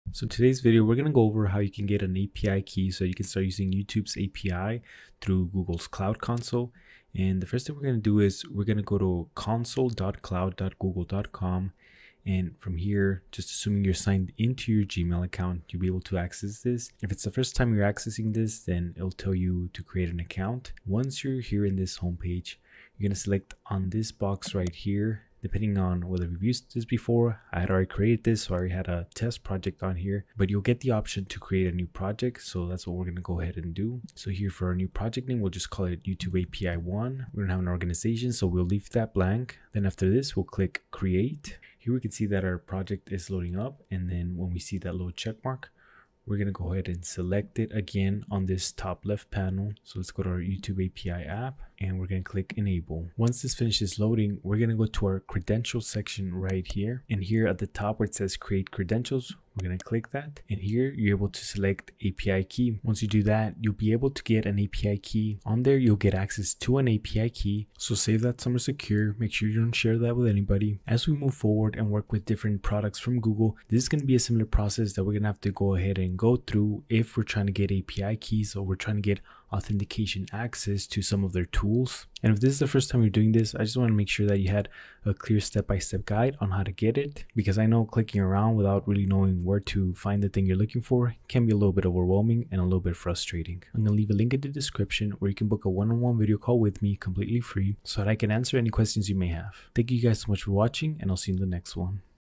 Requesting A Youtube API Key for Beginners
In this video we will be covering the basic steps needed to get a Youtube API key.

Skool Community (Beta, this is still in development, join now and get lifetime free access)